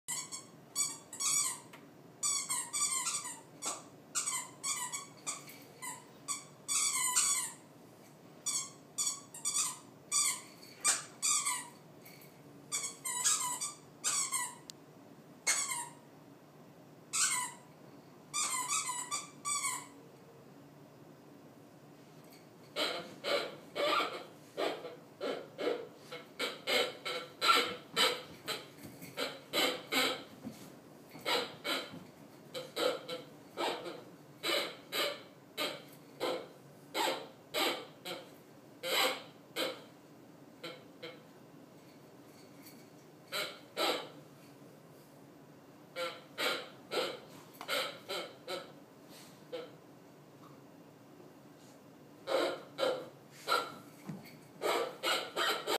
Ernie's latest toy!!!
Ernie playing with his new, green octopus that HE picked out at the store today!!!